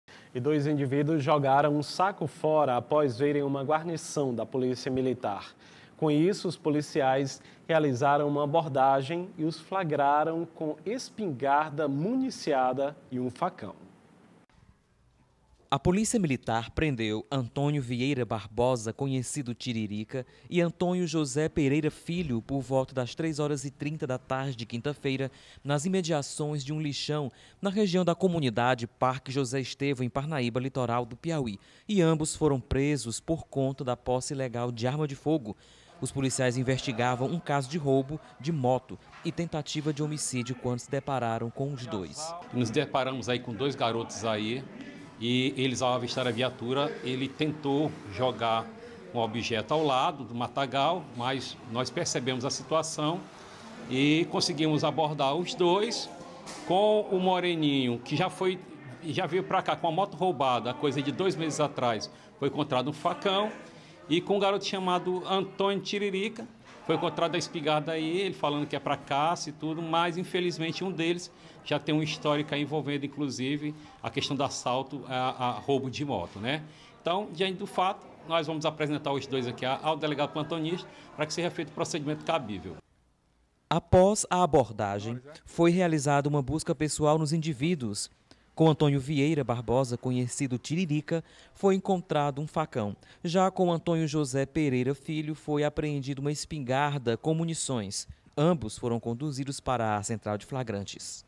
Polícia Militar prende indivíduos com arma de fogo após abordagem
Dois indivíduos foram flagrados a pé nas imediações de um lixão na tarde desta quinta-feira (23/04), na região da comunidade Parque José Estevão, em Parnaíba, litoral do Piauí, e ambos foram presos por conta de posse ilegal de arma de fogo. O flagrante aconteceu quando os policiais investigavam o caso de uma moto Honda/NXR 150 Bros, de cor laranja, placa OCH 8702, e os acusados tinham fugido da Polícia Militar após o abandono do veículo.